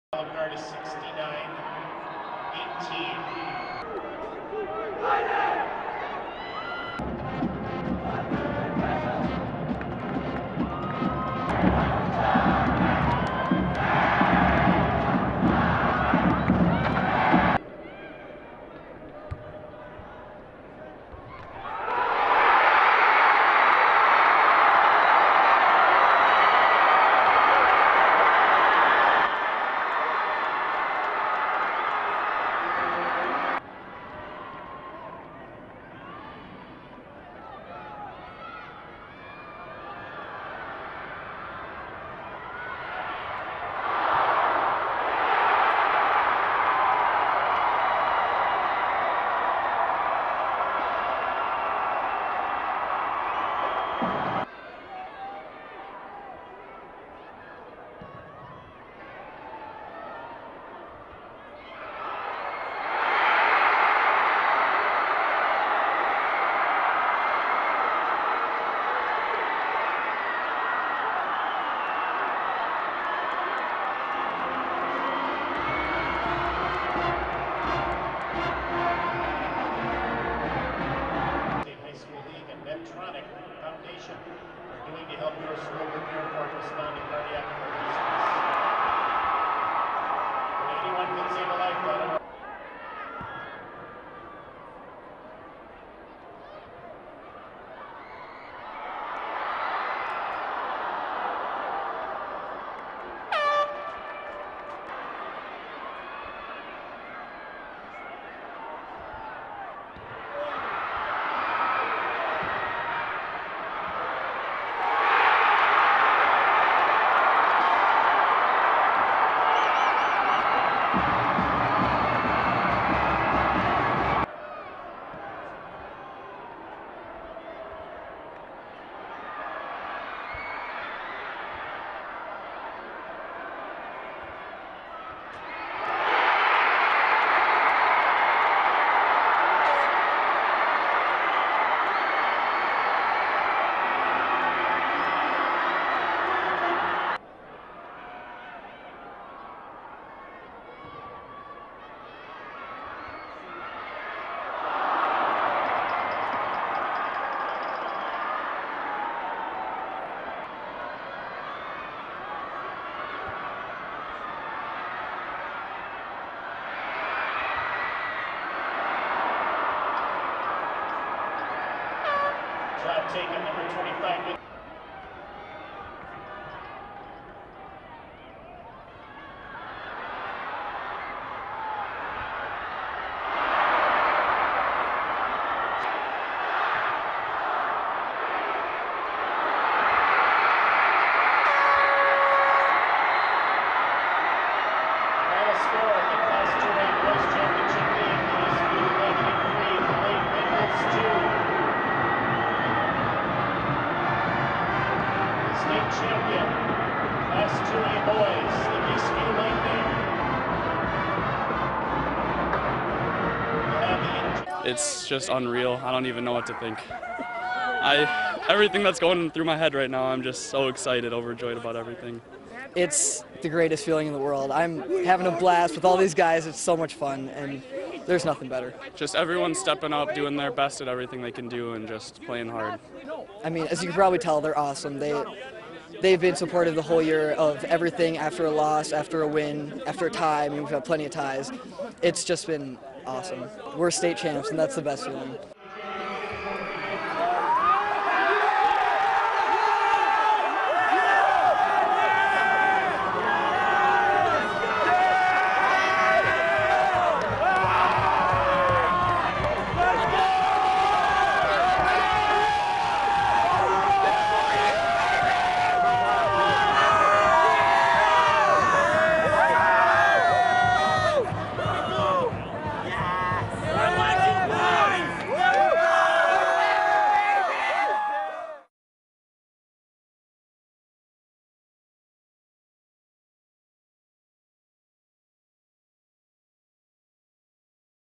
2012 MN Boys State Soccer Championship KSTP TV Highlights
Eastview 3, Blaine 2